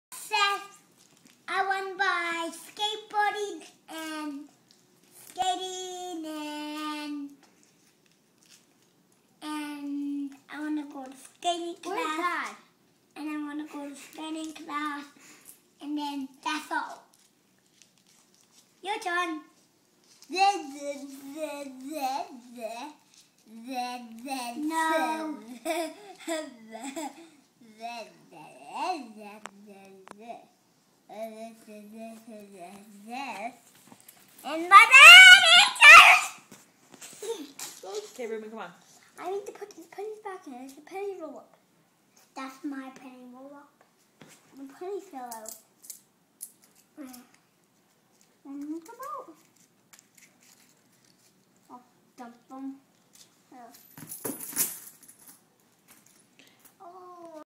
The Groot Boys.
Sending a video to their favorite American, Seth Davies. Jesse Fin was pumped on asking Seth to go skating, while Reuben was distracted with his "penny roller". these boys bring me so much joy. I love them